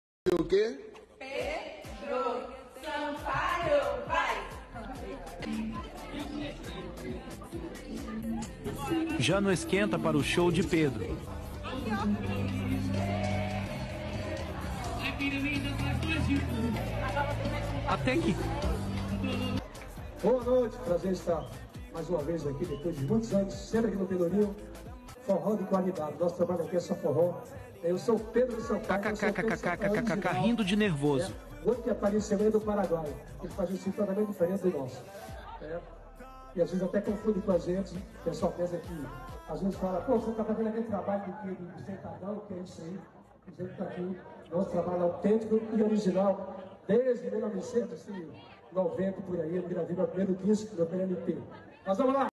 Confusão no show de Pedro Sampaio em Salvador
A galera não entendeu que era PedroS SampaioS kkkk